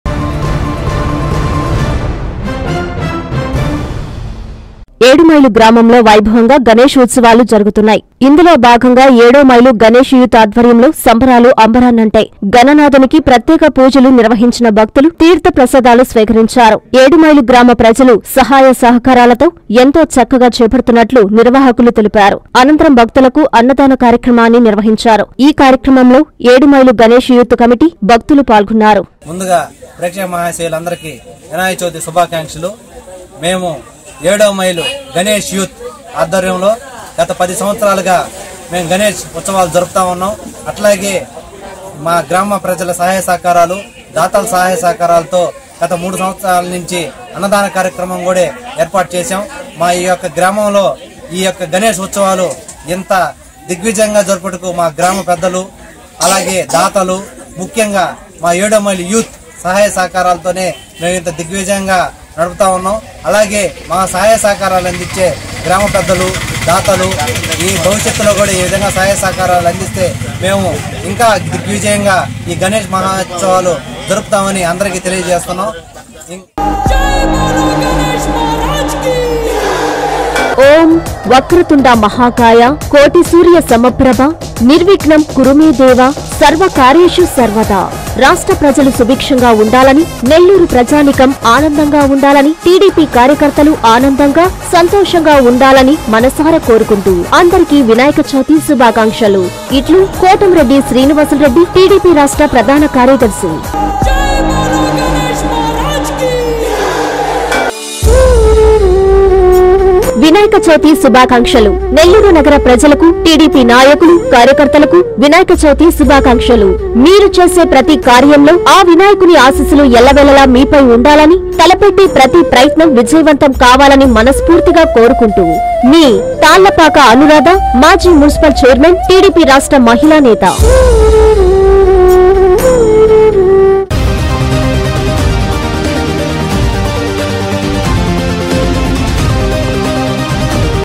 ఇందుకూరుపేట 7వ మైల్‌లో ఘ‌నంగా గ‌ణేష్ ఉత్స‌వాలు
- భ‌క్తిశ్ర‌ద్ధ‌ల‌తో పూజ‌లు నిర్వ‌హించిన భ‌క్తులు
- భ‌క్తుల‌కు అన్న‌దానం చేసిన 7వ మైలు గ‌ణేష్ యూత్ క‌మిటీ